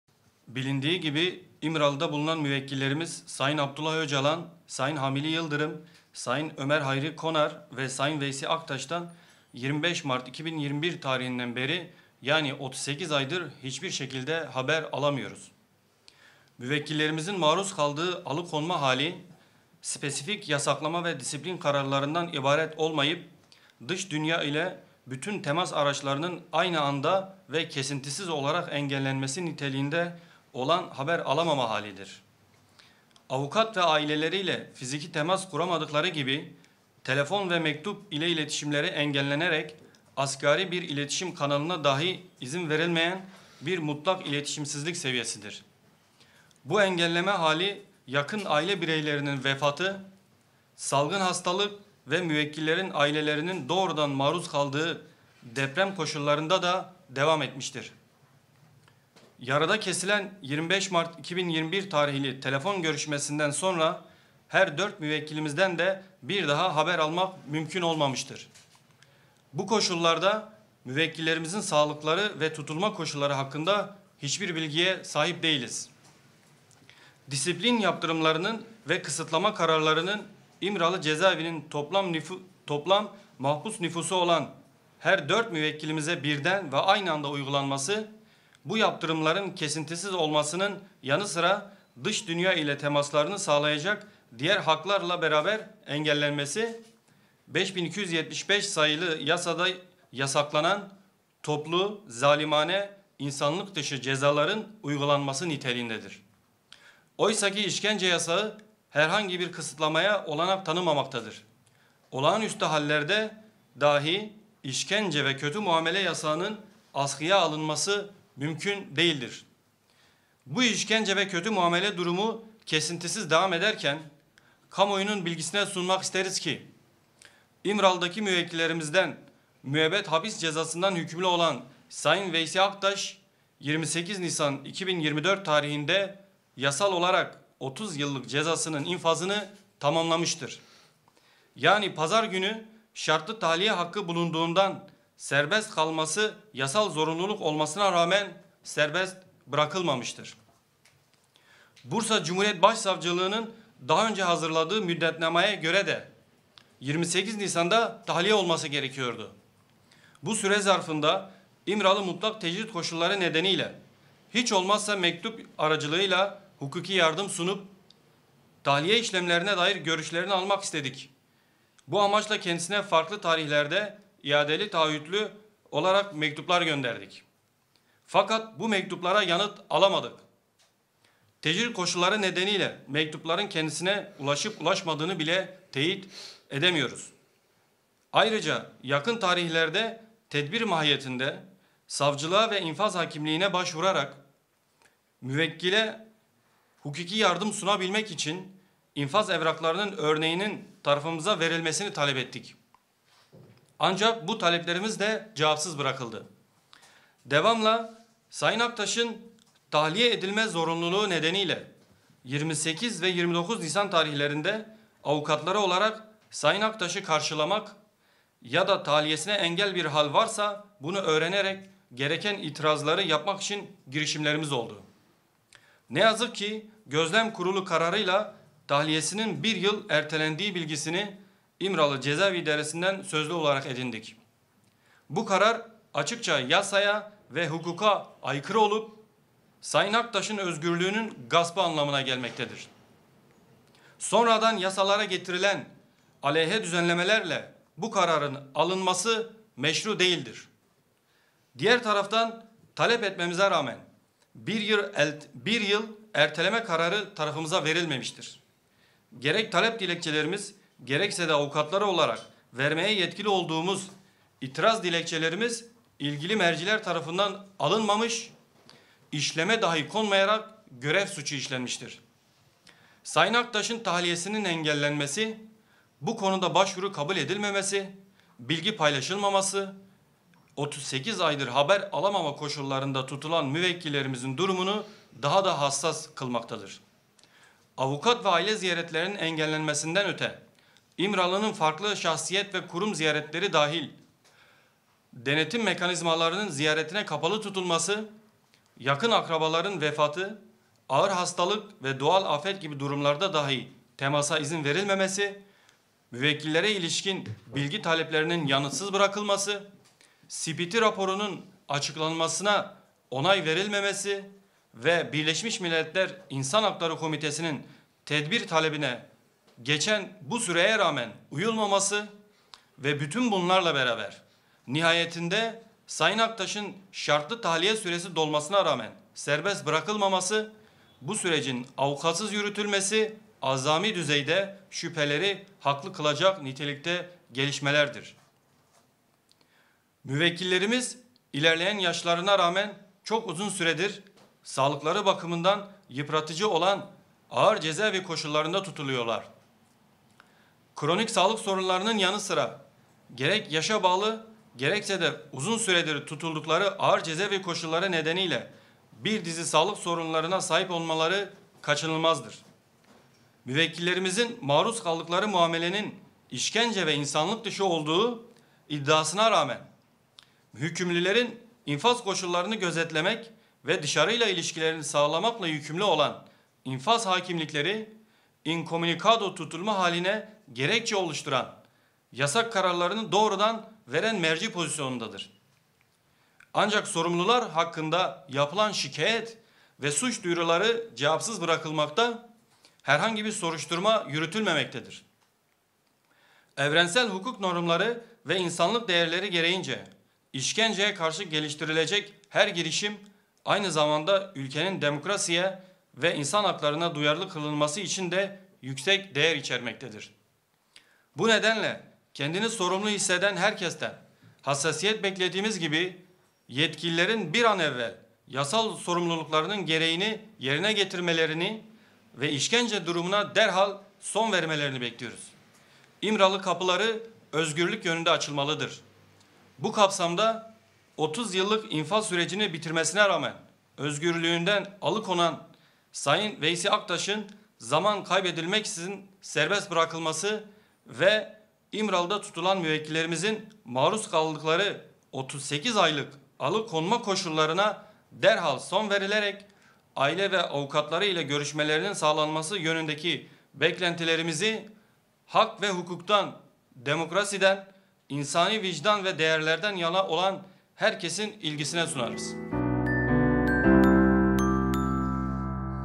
İMRALI’DA TUTULAN AKTAŞ’IN TAHLİYESİ 1 YIL ERTELENDİ
Asrın Hukuk Bürosu, İmralı’da PKK Lideri Abdullah Öcalan ile birlikte tutulan Veysi Aktaş’ın tahliyesinin 1 yıl ertelendiğini duyurdu. Açıklamada, "Yetkililerin bir an evvel işkence durumuna son vermelerini bekliyoruz. İmralı kapıları özgürlük yönünde açılmalıdır” denildi